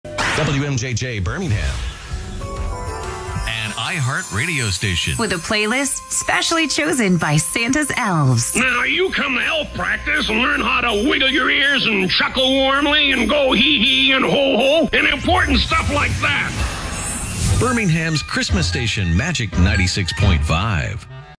WMJJ - Magic 96.5 - Station ID (4PM) November 28, 2022
The 4pm Station ID for WMJJ "Magic 96.5" Birmingham, Alabama. Copyright iHeartMedia.